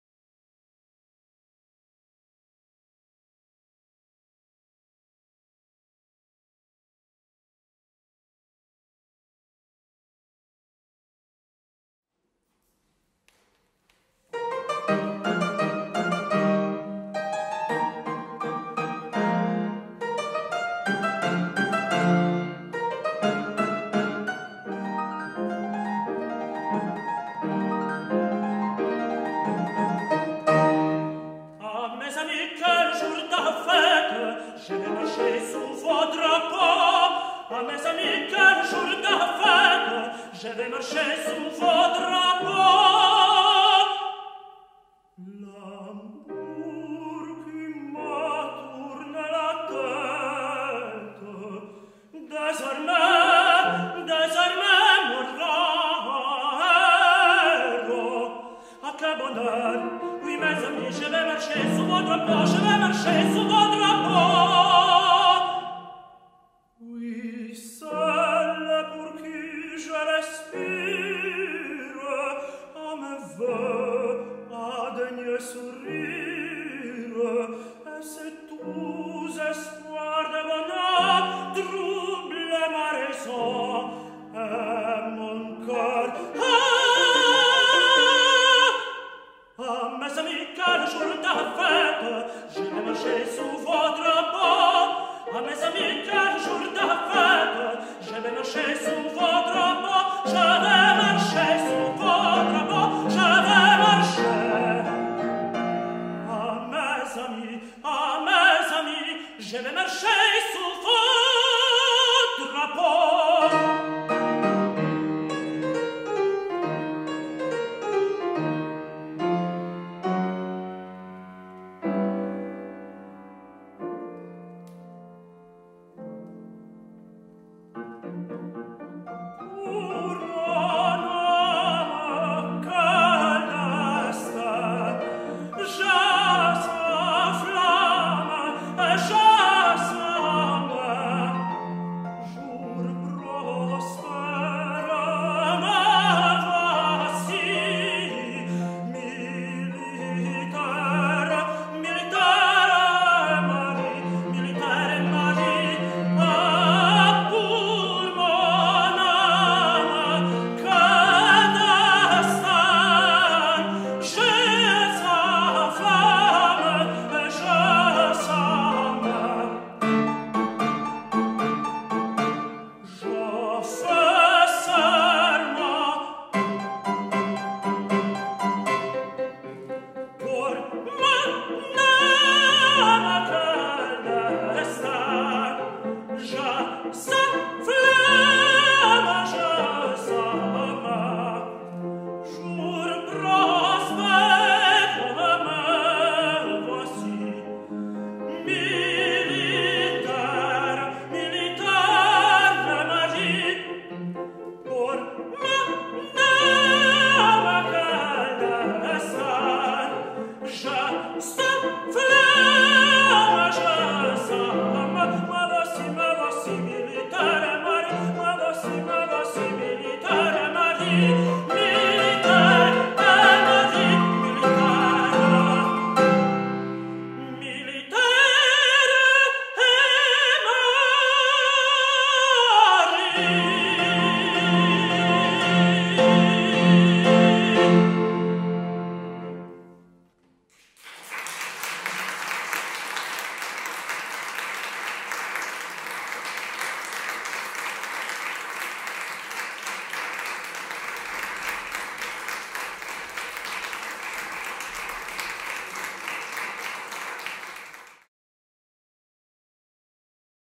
Stevan Karanac - Laureat Lav Mirski 2018 .
Lav Mirski 2018.
Stevan KARANAC - laureat natjecanja i pobjednik 5. kategorije
Ante BLAŽEVIĆ - klavirska pratnja